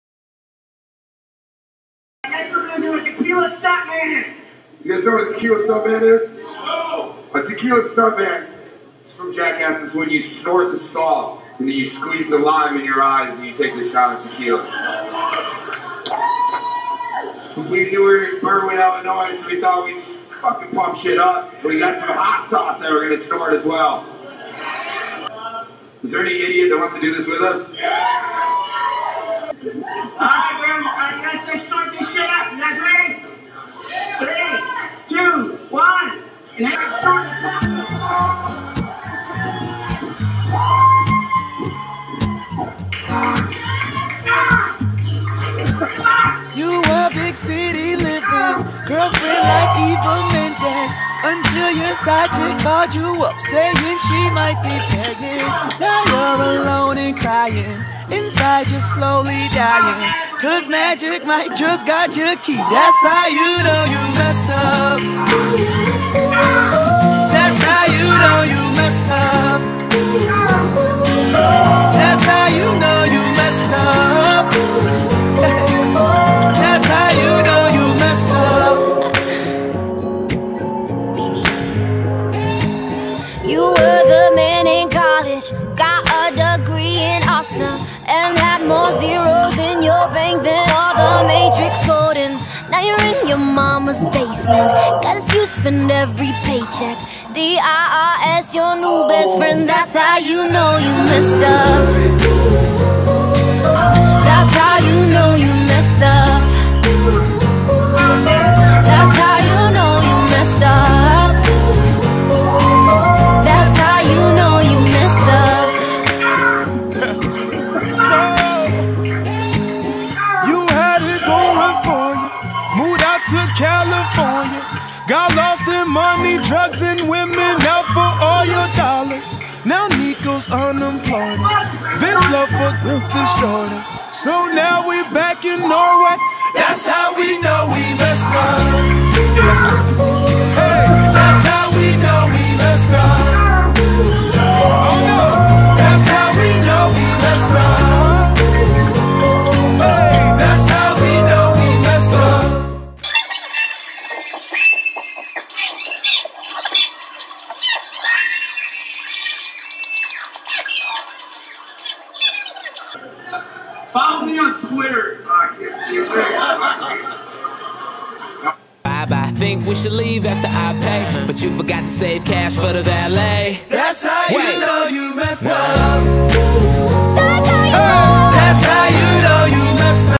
The Tequila Stuntman with Preston lacy from jackass with Tom Garland Mike Stricker Tim Brennan
I (Jake Olsen) the guy in the Conan tshirt did the tequila stuntman along with a few other fans at the Preston Lacy comedy show, it was quite a bit of fun